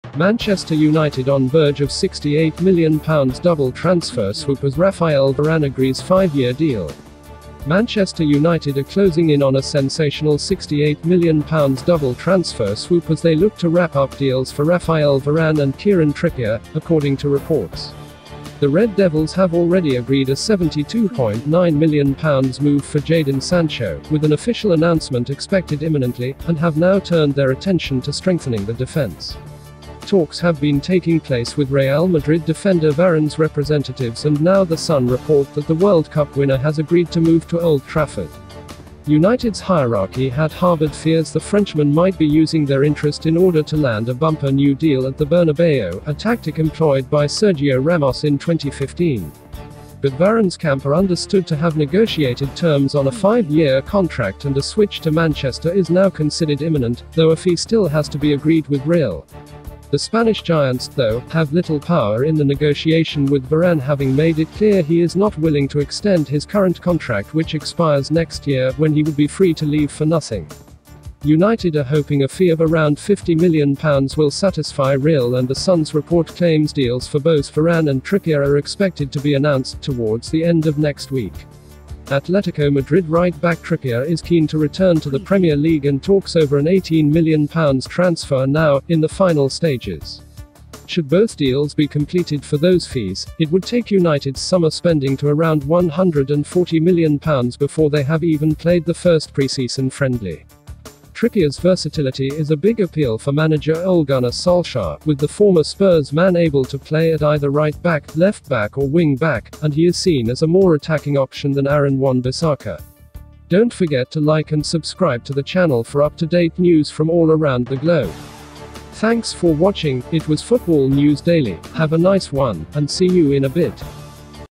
Manchester United close to £68m double transfer
Manchester United on verge of £68m double transfer swoop as Raphael Varane agrees five-year deal

Manchester United are closing in on a sensational £68million double transfer swoop as they look to wrap up deals for Raphael Varane and Kieran Trippier, according to reports.

The Red Devils have already agreed a £72.9million move for Jadon Sancho – with an official announcement expected imminently – and have now turned their attention to strengthening their defence.

Talks have been taking place with Real Madrid defender Varane’s representatives and now The Sun report that the World Cup winner has agreed to move to Old Trafford

United’s hierarchy had harboured fears the Frenchman might be using their interest in order to land a bumper new deal at the Bernabeu – a tactic employed by Sergio Ramos in 2015.

But Varane’s camp are understood to have negotiated terms on a five-year contract and a switch to Manchester is now considered imminent, though a fee still has to be agreed with Real.

The Spanish giants, though, have little power in the negotiation with Varane having made it clear he is not willing to extend his current contract which expires next year – when he would be free to leave for nothing

United are hoping a fee of around £50million will satisfy Real and The Sun’s report claims deals for both Varane and Trippier are expected to be announced ‘towards the end of next week’.

Atletico Madrid right-back Trippier is keen to return to the Premier League and talks over an £18m transfer are now ‘in the final stages’.

Should both deals be completed for those fees, it would take United’s summer spending to around £140m before they have even played their first pre-season friendly.

Trippier’s versatility is a big appeal for manager Ole Gunnar Solskjaer – with the former Spurs man able to play at either right-back, left-back or wing-back – and he is seen as a more attacking option than Aaron Wan-Bissaka.